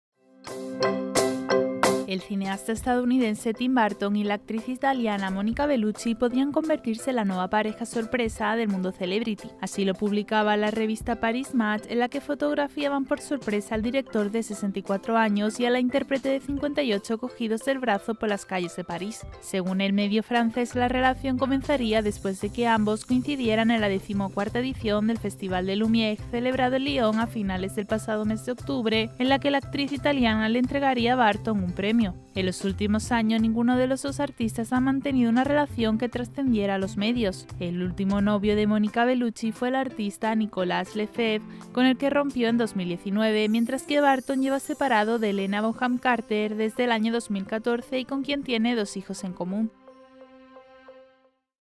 Tim Burton y Mónica Belluci, nueva pareja sorpresa
El cineasta estadounidense Tim Burton y la actriz italiana Monica Bellucci podrían convertirse en la nueva pareja sorpresa del mundo celebrity. Así lo ha publicado la revista París Match en la que fotografiaban por sorpresa al director, de 64 años,  y a la intérprete, de 58, cogidos del brazo por las calles de París.

(Fuente: Paris Match/ Instagram/ Europa Press)